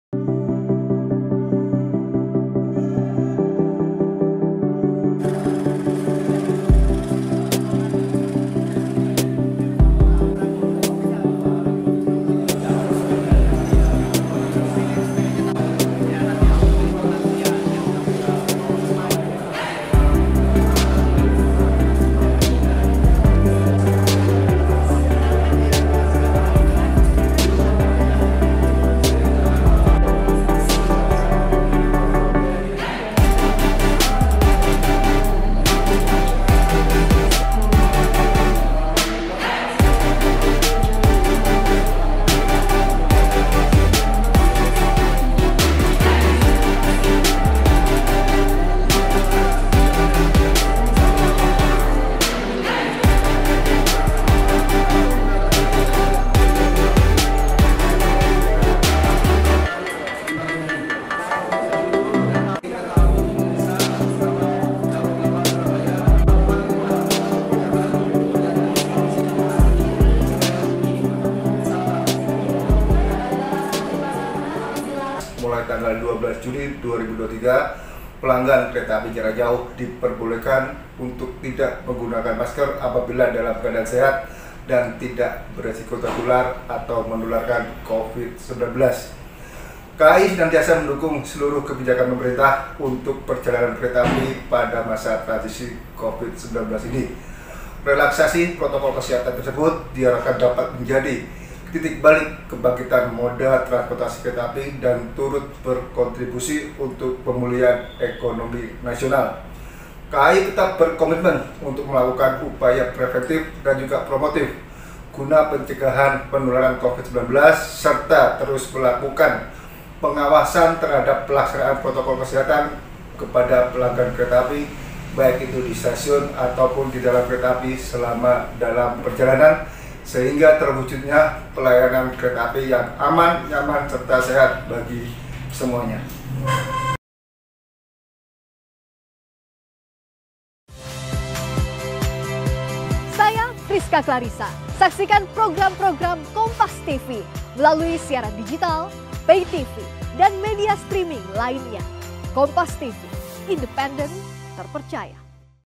Sekarang Penumpang Kereta Api Boleh Tak Pakai Masker, Asal....
SURABAYA, KOMPAS.TV  -   Penumpang kereta api boleh tak pakai masker, asal sehat.

Mengacu pada Surat Edaran Kementerian Perhubungan Nomor 17 Tahun 2023 tentang Protokol Kesehatan Pelaku Perjalanan Orang dengan Transportasi Kereta Api pada Masa Transisi Endemi Covid-19.

PT KAI tidak mewajibkan penumpang memakai masker, ketentuan ini berlaku sejak 12 juni kemarin.

Kendati deimikan, penumpang yang sakit dan berisiko tertular tetap diwajibkan memakai masker. (*)

Kompas TV
Independen | Terpercaya